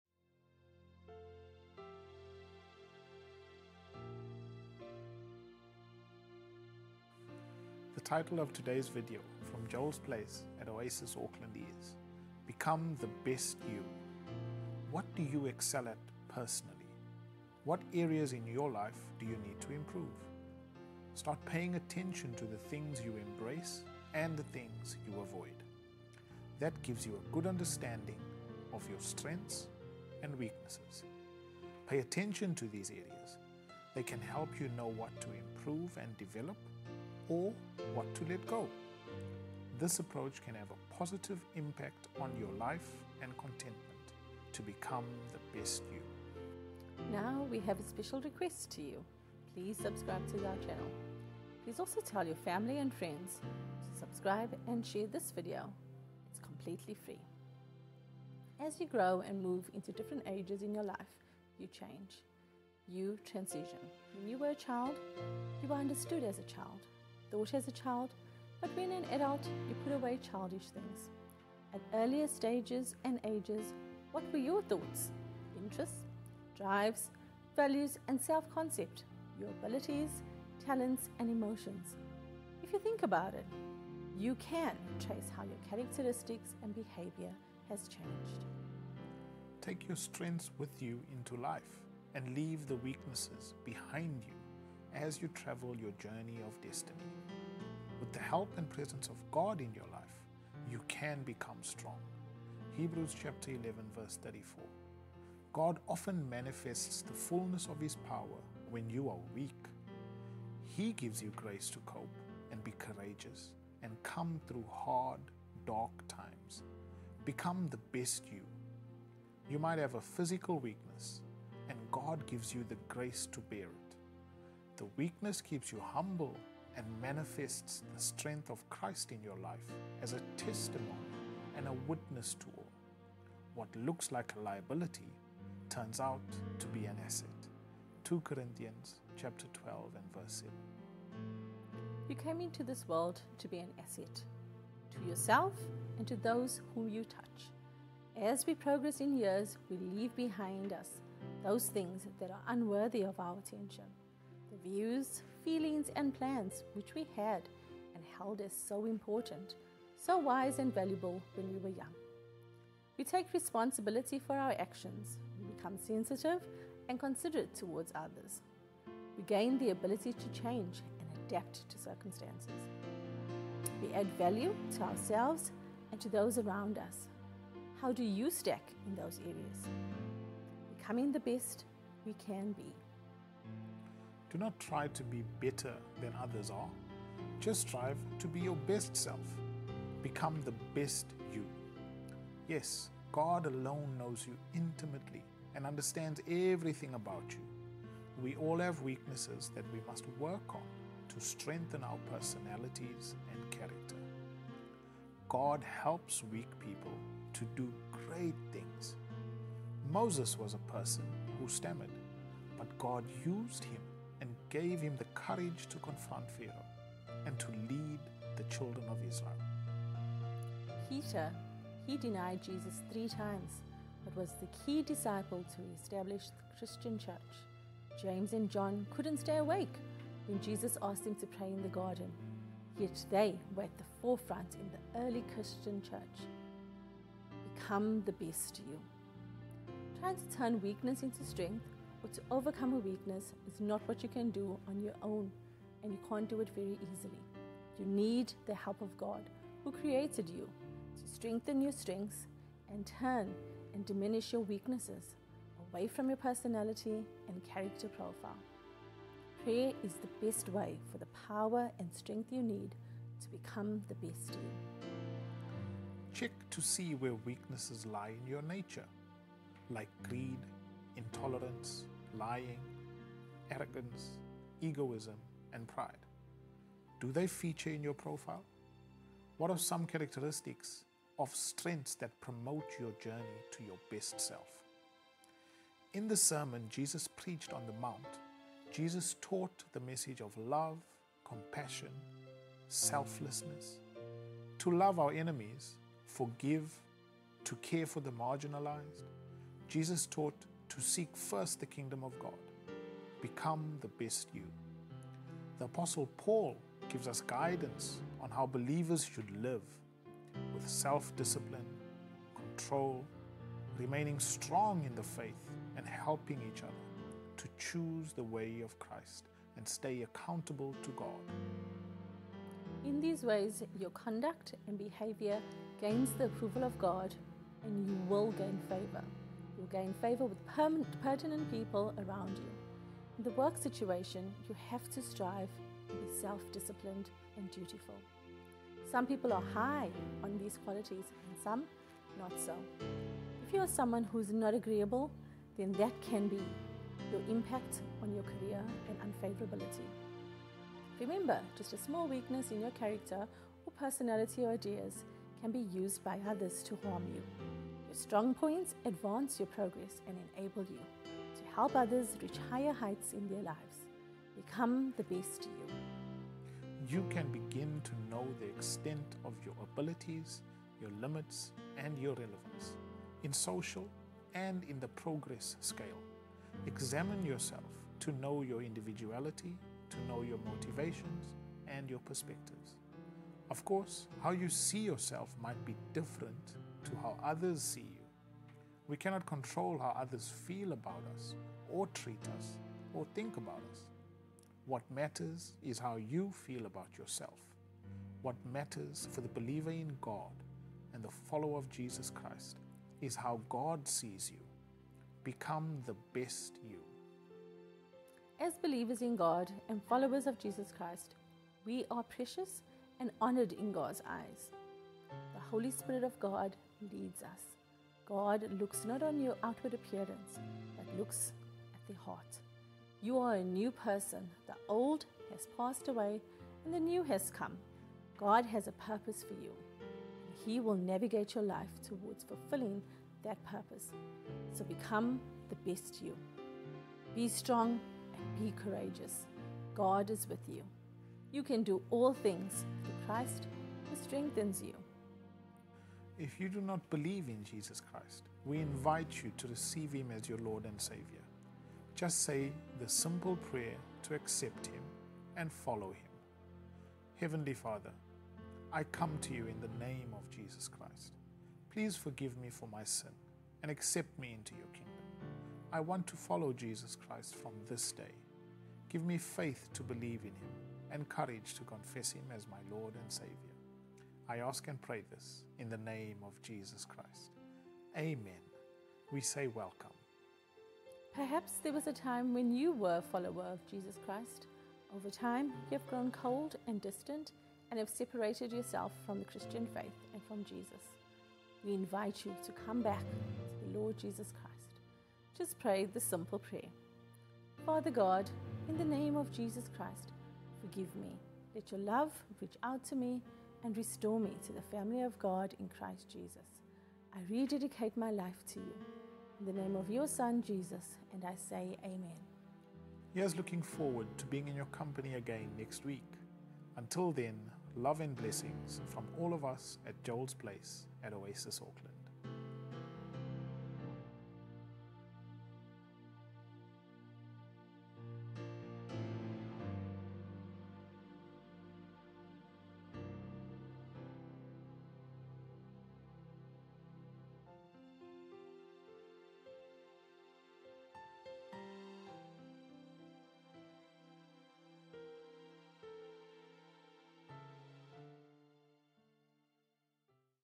Become the best you
In this inspiring video from Joel’s Place at Oasis Auckland, titled "Become the Best You," Brad and Claudine guide viewers through the essential process of self-reflection and personal development. They encourage individuals to identify their strengths and weaknesses, which is crucial for fostering growth and fulfillment. The discussion also touches on the evolution of our thoughts and values as we progress through different life stages, highlighting the importance of spiritual support in navigating life's challenges. This video is a valuable resource for anyone looking to enhance their life journey.